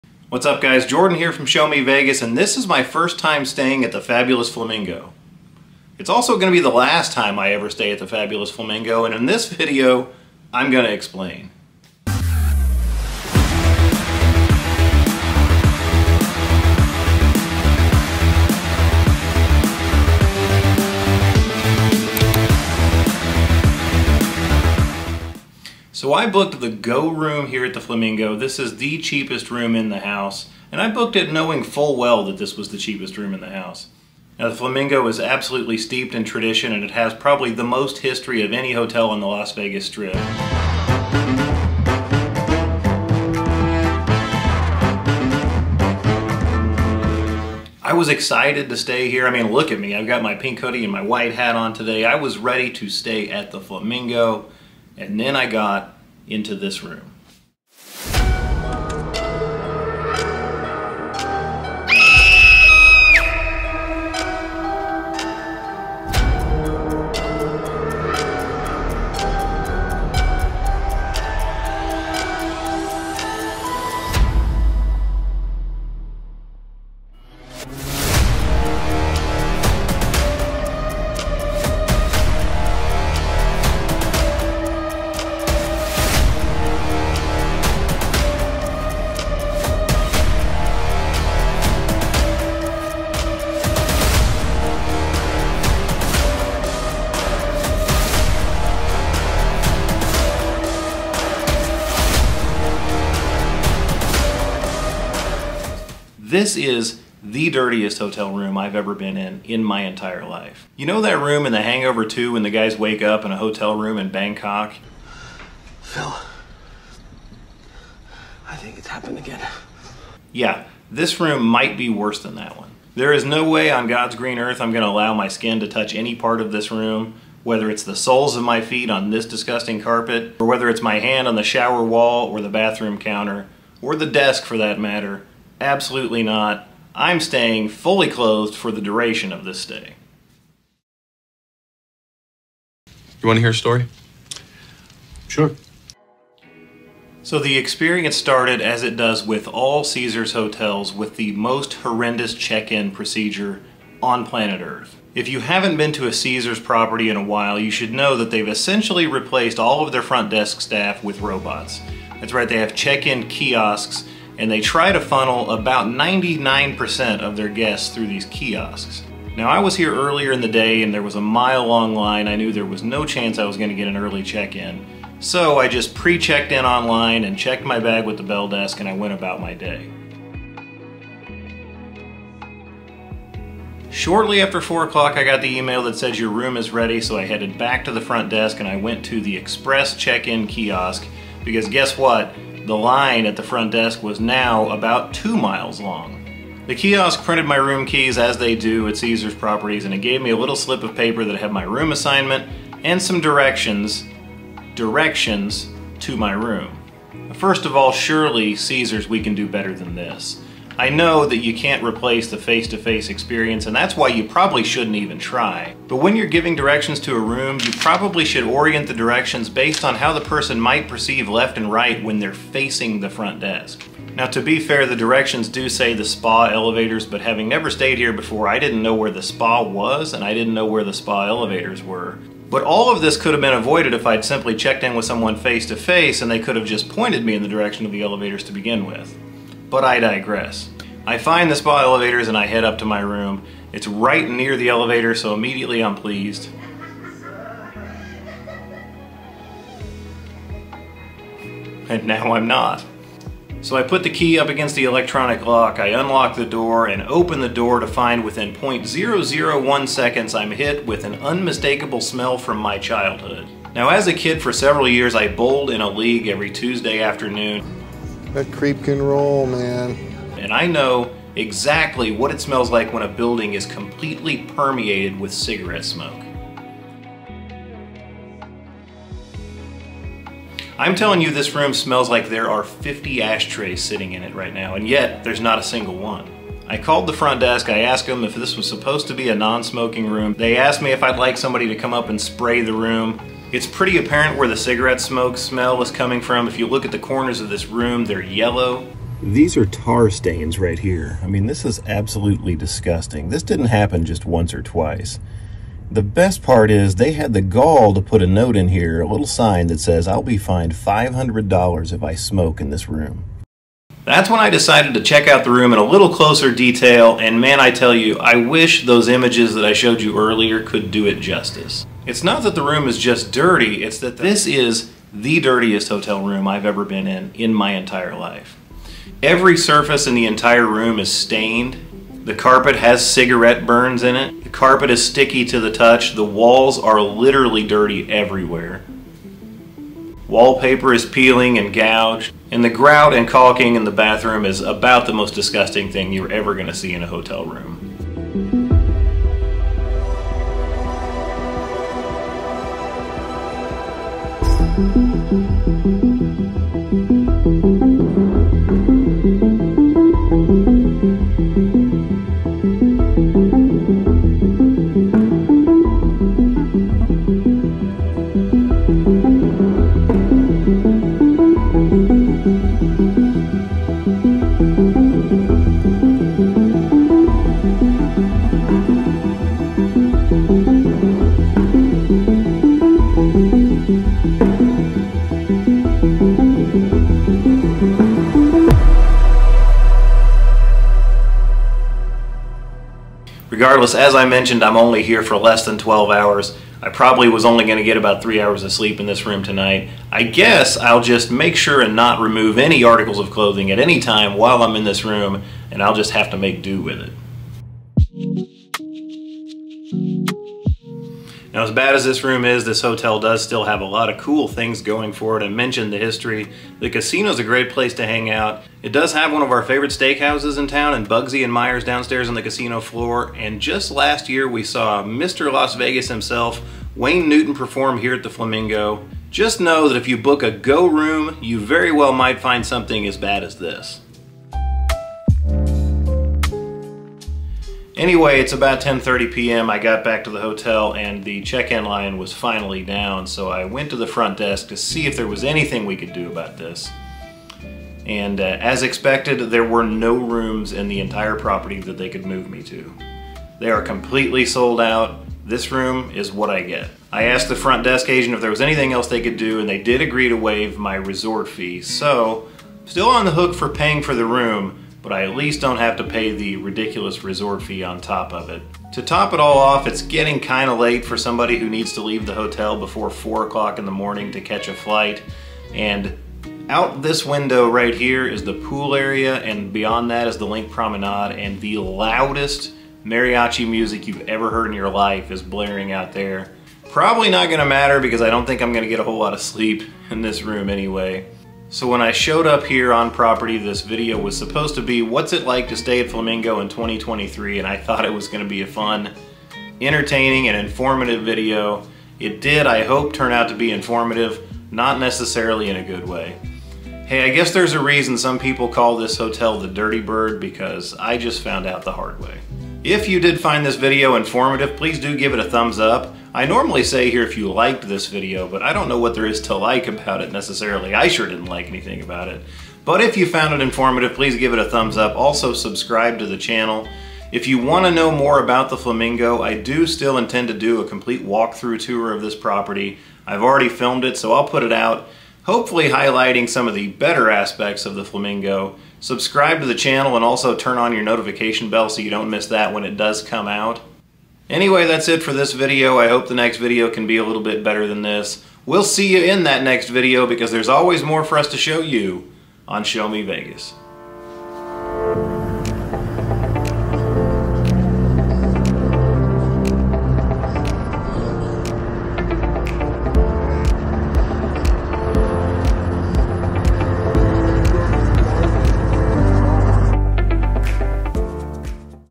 DISGUSTING Flamingo Go Room is the WORST Vegas Hotel Room I’ve ever seen! DIRTY BIRD Las Vegas 2023
Jordan just found out the HARD WAY why some people refer to the Flamingo Las Vegas as “the dirty bird.”  First and LAST time staying at Bugsy Seigel’s place…